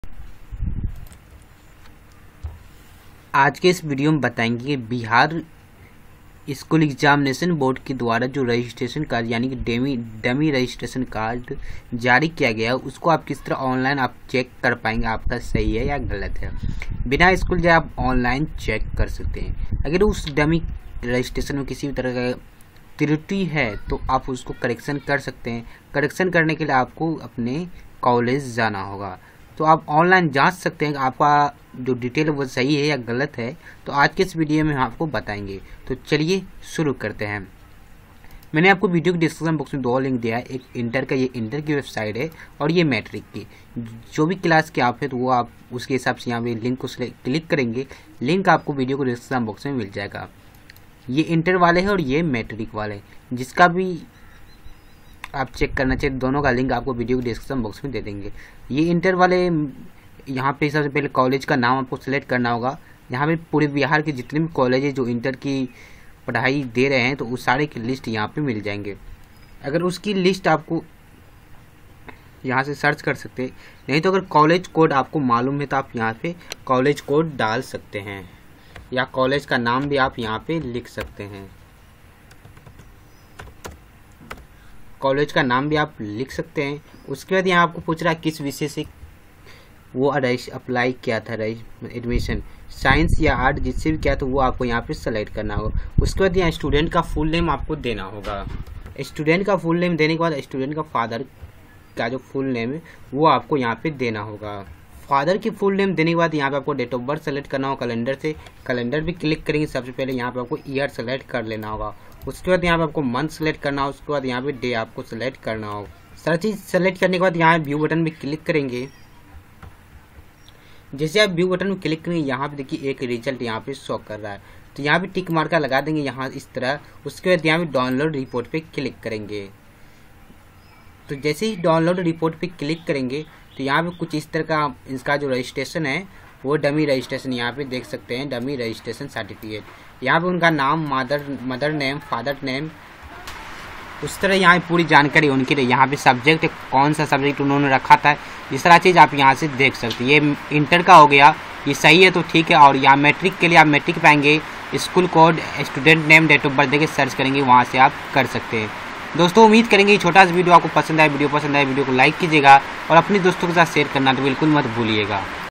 Bihar Inter, Matric Dummy Registration Certificate Online Download Kare | Zeeshan Monitor
THANKS FOR WATCHING